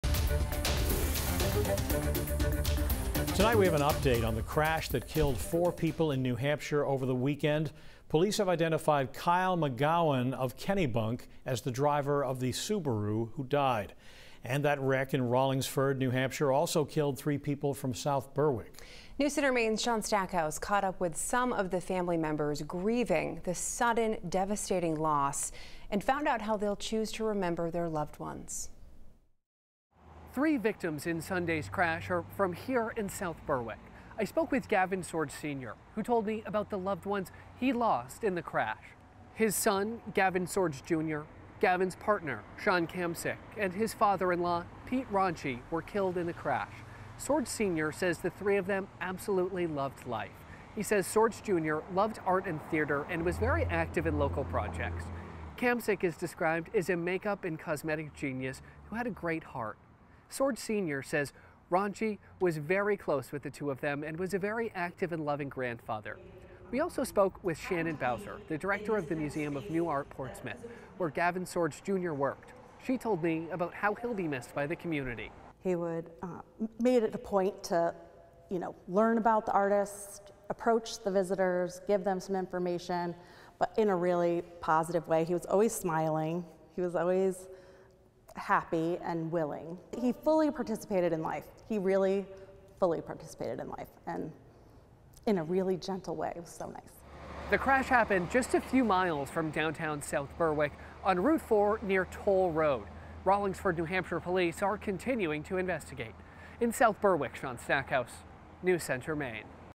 Loved ones remember Maine victims of New Hampshire crash
Peter Ronchi, 58, Gavin Sorge Jr., 22, and Sean Kamszik, 23, all of South Berwick, were killed in the crash.